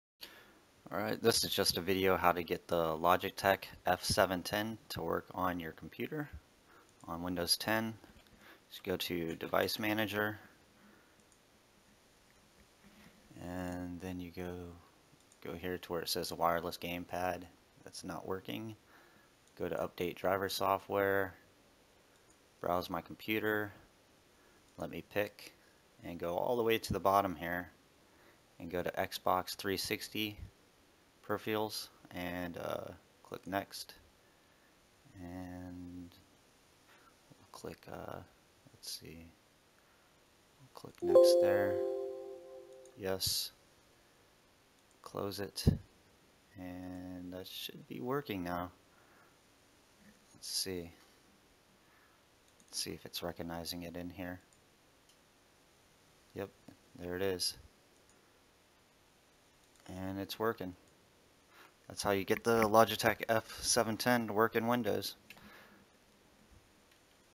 How To Get The Logitech Gamepad Controller f710 To Work In Windows
Windows Got You Down? Can't get Logitech controller to work in windows? Here's how!